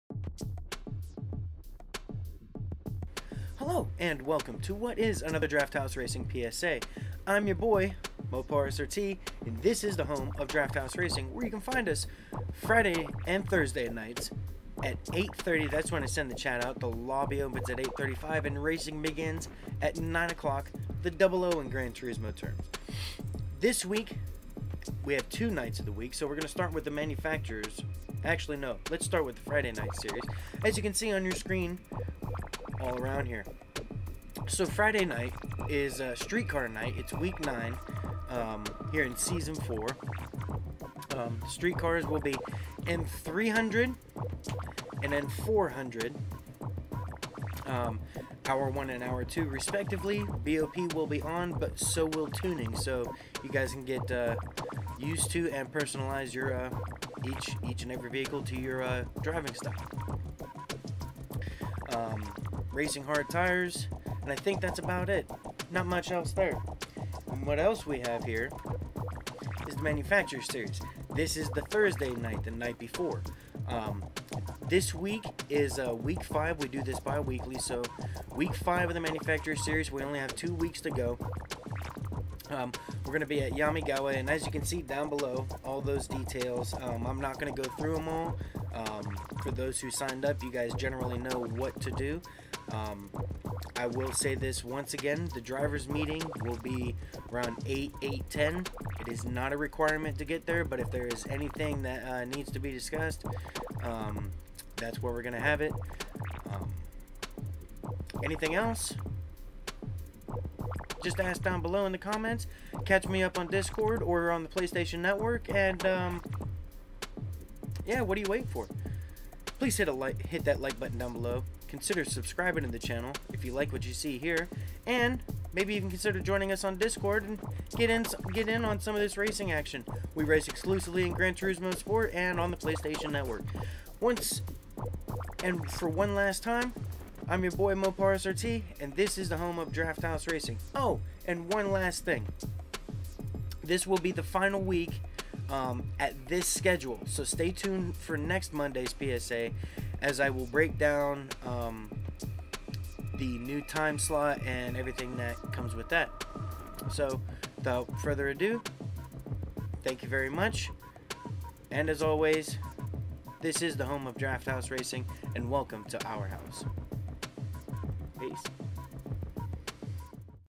DHR Season 4 PSA - Week 5/10
Aloha and Welcome!!! Its me, ya boy MoparSRT!!
Join us on Discord, Facebook and add me as a friend on PSN to join the DHR Chat
Playstation - MoparSRT
Welcome to the Season 4... Week 9
This Week - Street Car Night
Chat open at 8:30 pm eastern
Lobby Opens - 8:35 pm
Racing starts at 9 with 5-6 minute breaks between races
Class - Street Car / N300-N400
Track - Sardegna Road B
Tires - Racing Hard
BoP - On
Tuning - On
Manufacturer's Series
Week - 5
Qualifying - 8:55pm
Racing - 9:00pm
Race 1 - N400 - 15 Minutes
10 Minutes intermission
Race 2 - Group 4 - 20 Minutes - Fuel - 7x
10 Minute Intermission
Race 3 - Group 3 - Tire - 7x + Fuel - 4x

All Times are Eastern USA and Settings are Subject to change